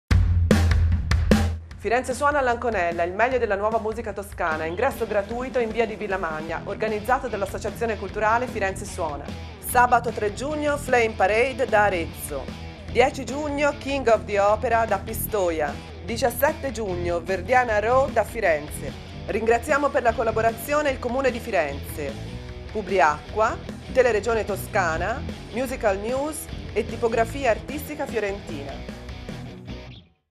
Promo Firenze Suona All'Anconella
Firenze Suona All'Anconella, il meglio della nuova musica Toscana, ingresso gratuito in Via di Villamagna, organizzato dall'associazione culturale Firenze Suona.
In collaborazione con:
Comune Di Firenze
Publiacqua
Teleregione Toscana
MusicalNews.com
Tipografia Artistica Fiorentina